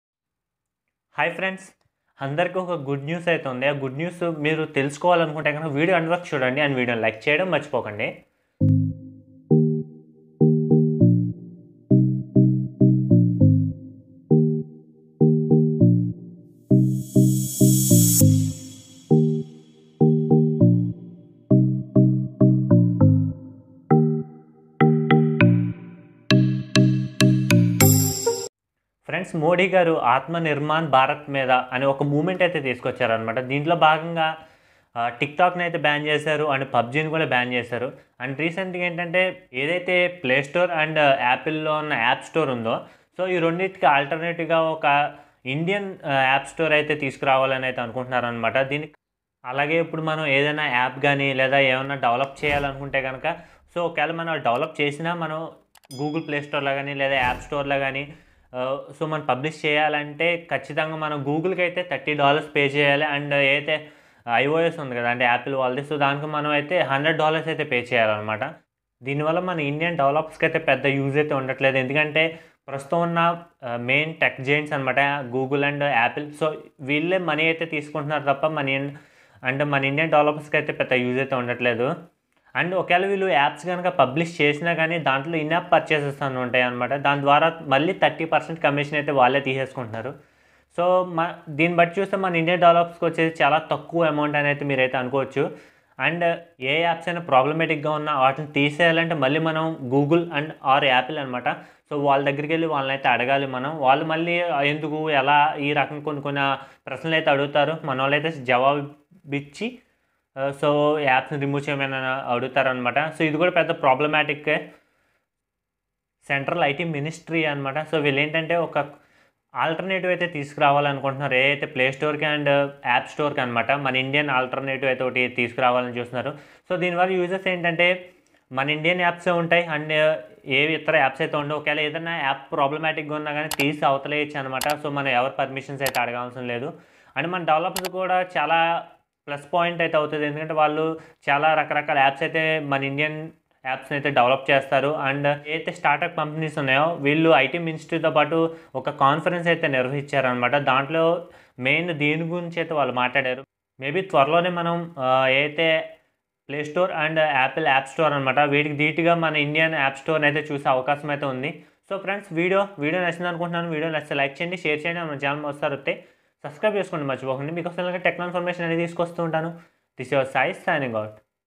Breaking news-Indian play store is coming😃😃||bye Google play store and app store|తెలుగులో
Friends here I gave full information about Indian playstore and its details.
For more details watch our video here.